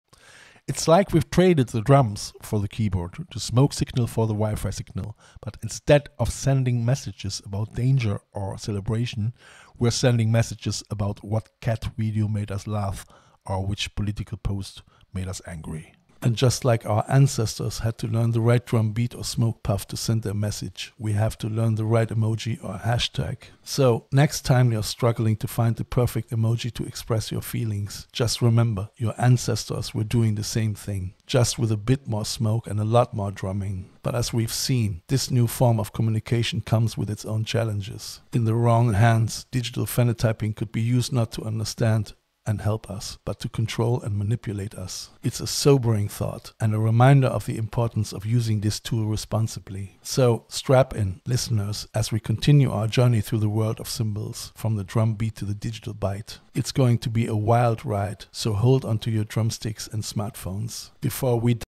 Arteetude 185: Drumbeats to Data - Tracing the Rhythm of Communication with Detlef Schlich
"Arteetude 185: Deciphering Digital Phenotyping - A Leap from Ancient Symbols to Modern Ethics with Detlef Schlich"
In this episode, Detlef Schlich takes us on a journey through the digital landscape, exploring the ethical implications of digital phenotyping.